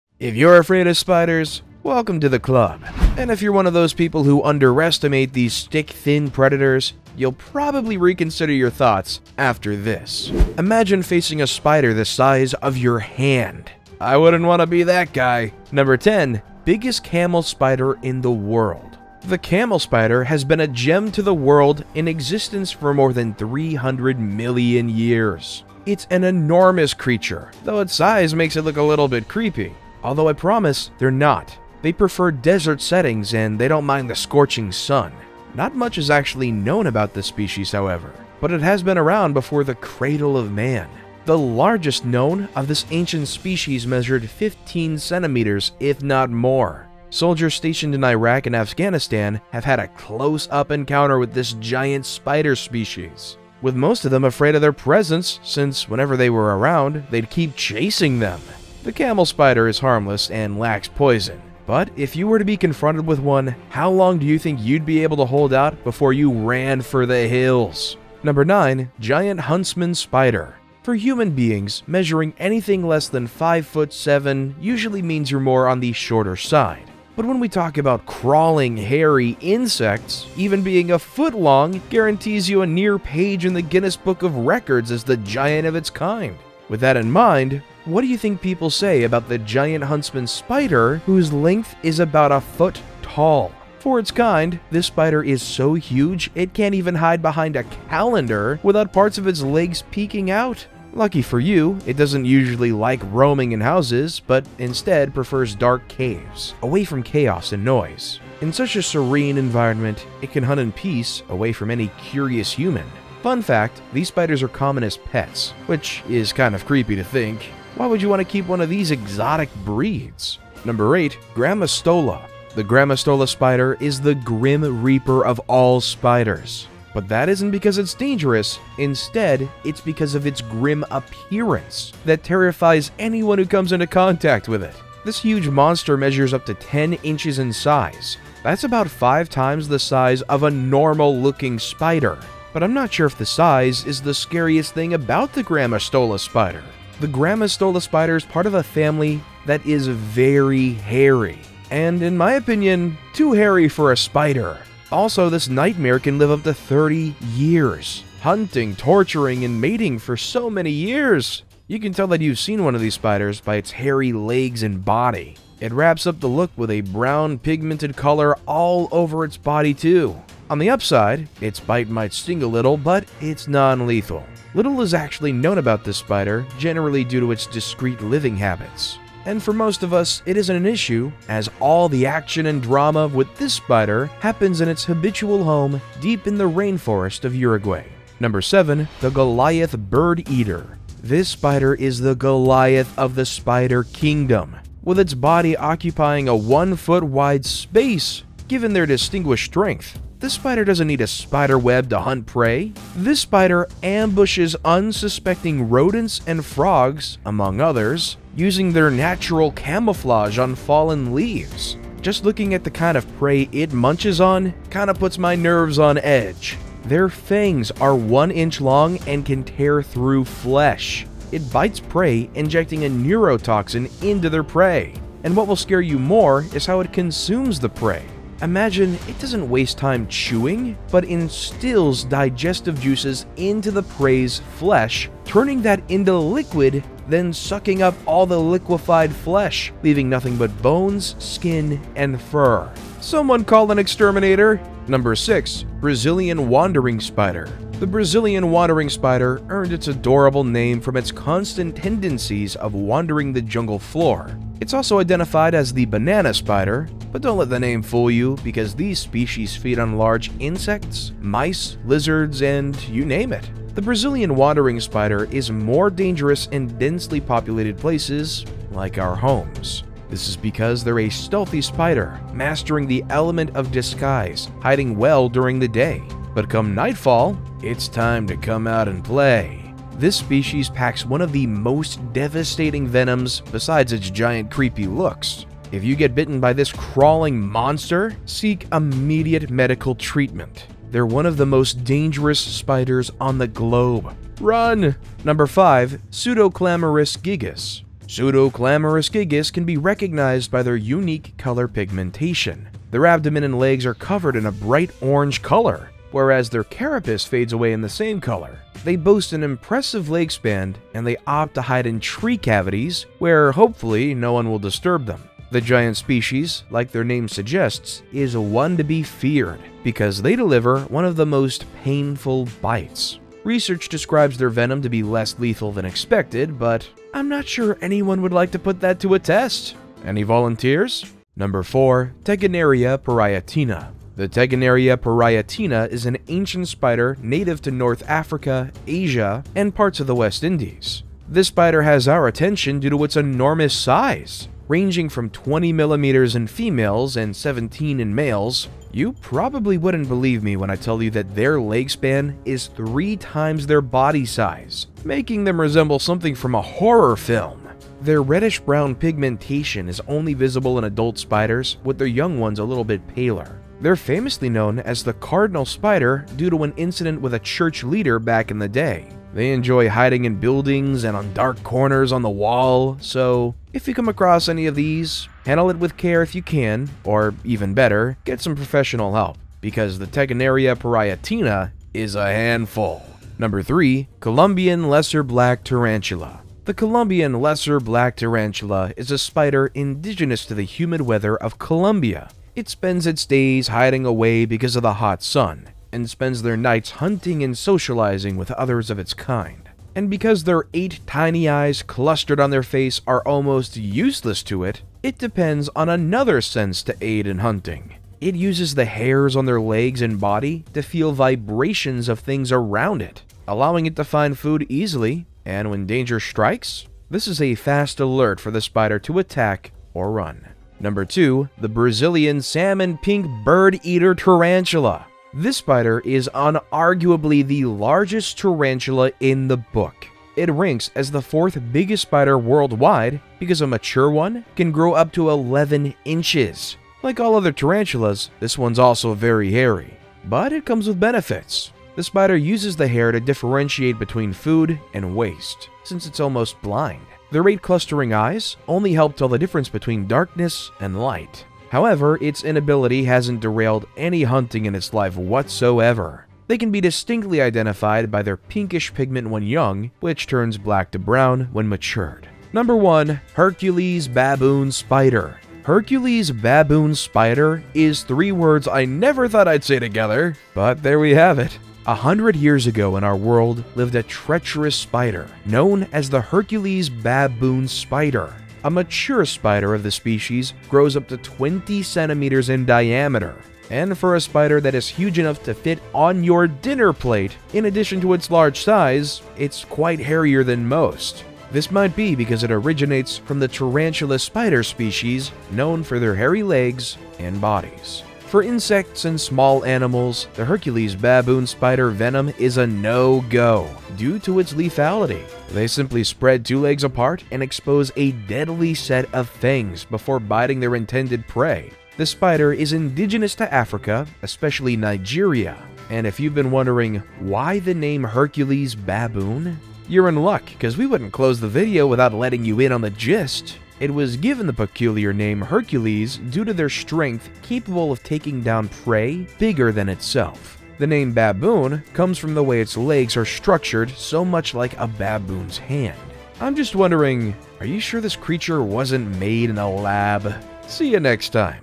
Top 10 Biggest Spiders In The World
If you’re afraid of spiders, I'd recommend you keep a safe distance from what we’re about to dive into. If you are one of those guys who undermine these stick-thin predators, you'd probably reconsider your thoughts after this. Imagine facing a spider the size of your hand! I wouldn’t wanna be that guy!!

Chapters:

00:00 Intro
00:16 Biggest Camel Spider in the World
01:08 Giant Huntsman Spider
02:00 Grammostolla
03:08 The Goliath bird eater
04:05 Brazilian wandering spider
04:56 Pseudoclamoris gigas
05:33 Tegenaria parietina
06:23 Colombian lesserblack tarantula
07:02 The Brazilian salmon pink bird eater tarantula
07:42 Hercules baboon spider